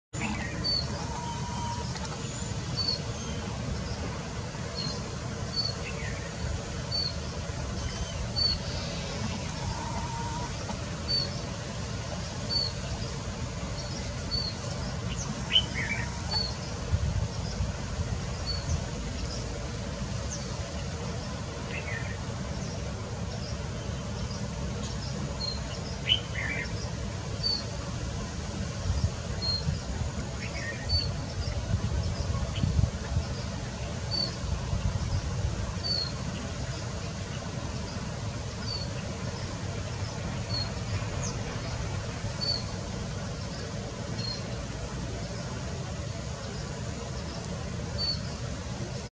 Eurasian Sparrowhawk (Accipiter nisus) Spotted in India
Eurasian sparrowhawk species is wholly migratory, wintering from India eastwards through Southeast Asia. Accipiter nisus are also known as the northern sparrowhawk or simply the sparrowhawk.